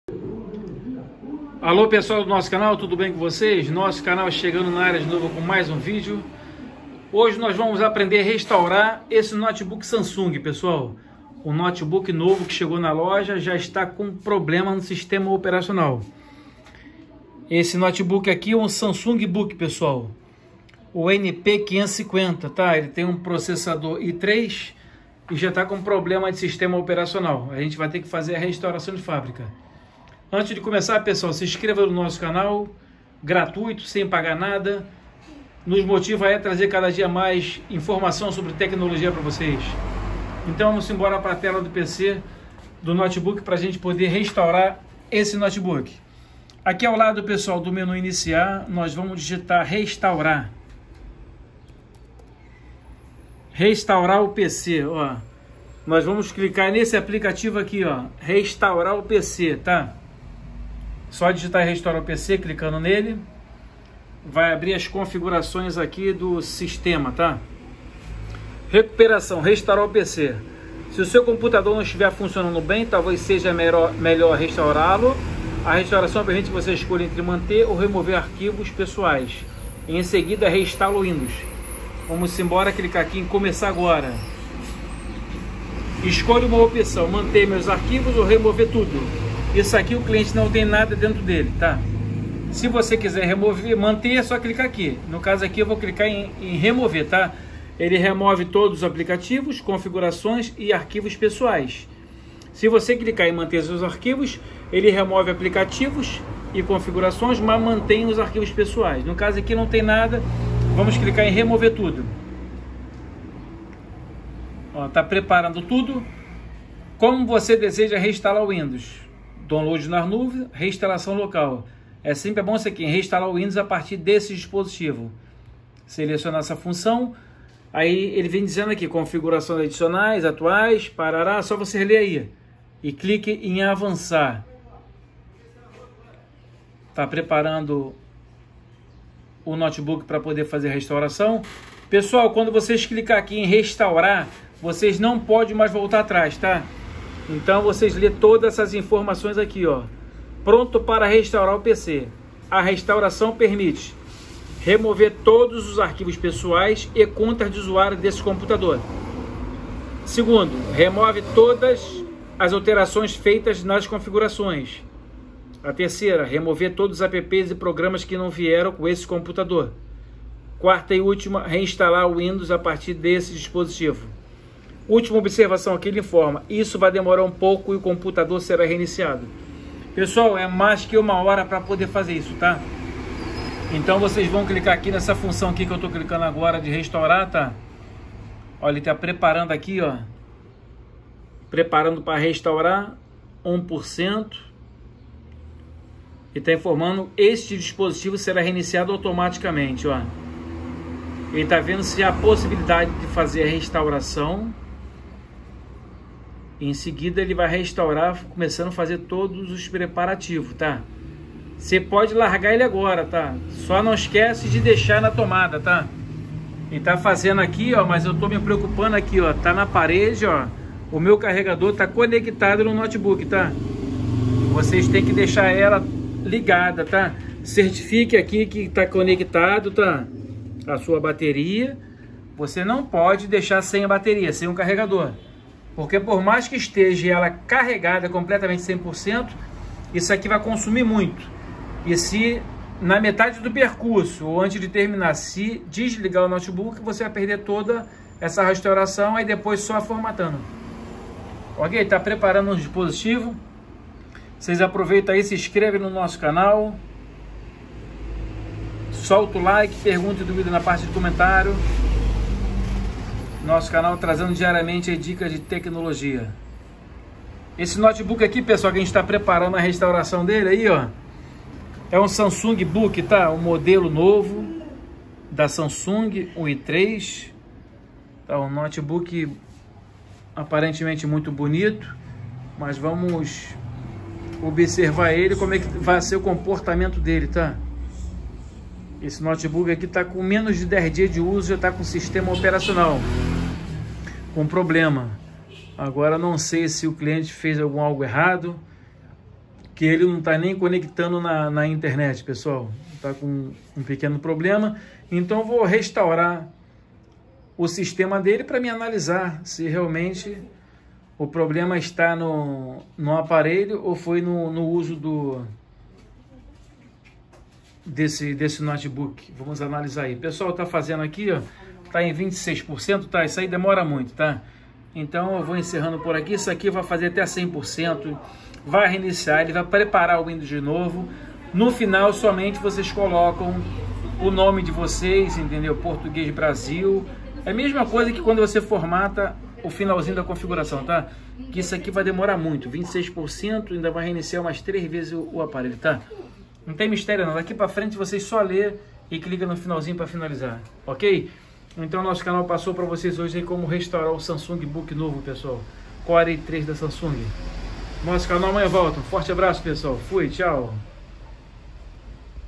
Como Restaurar Samsung Book para as Configurações de Fábrica
Vamos aprender tudo no passo a passo como restaurar de um forma simples e correta, mantendo todos os seus arquivos